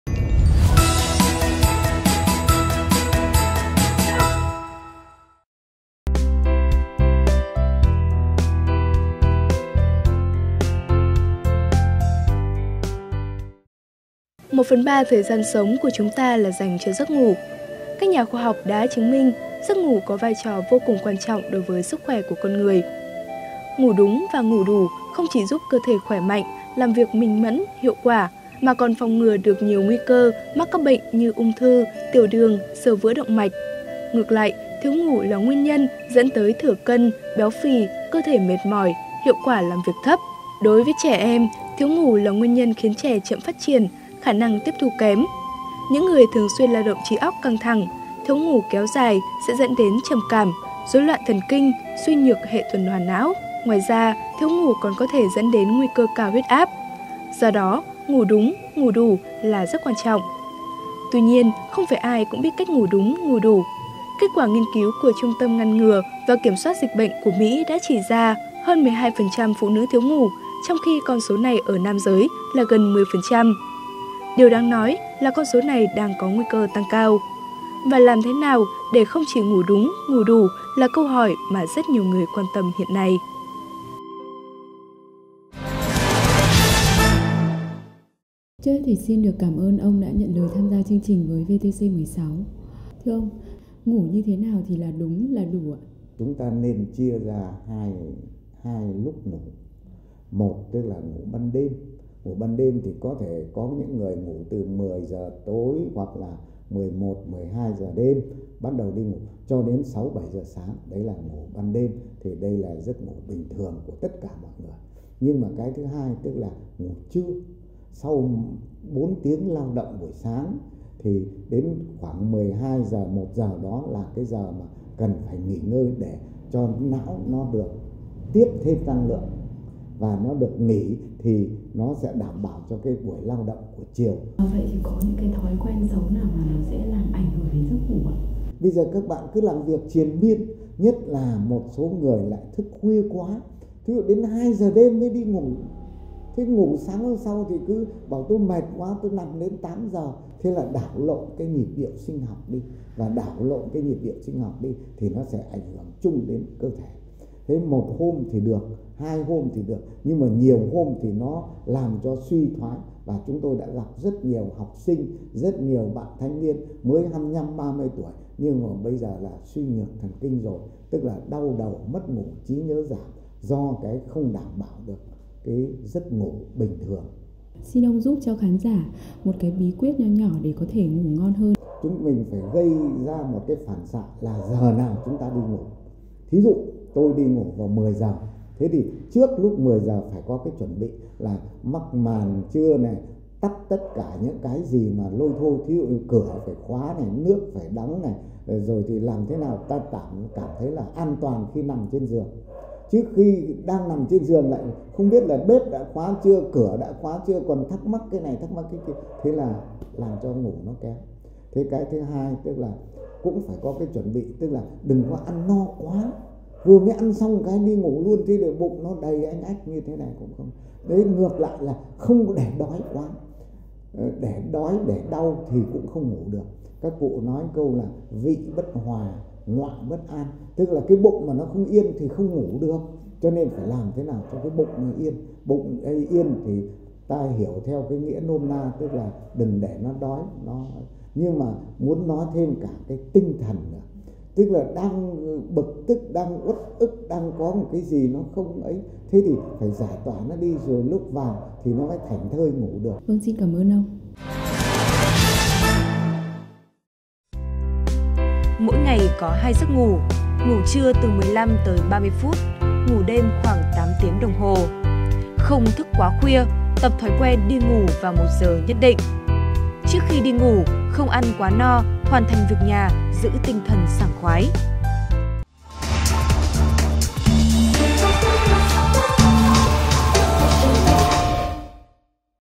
Ngủ thế nào cho đúng và đủ? | VTC
VTC | Giấc ngủ được coi là một “liều thuốc tiên” vô giá đối với sức khoẻ. Làm thế nào để ngủ đúng, ngủ đủ để đảm bảo sức khoẻ?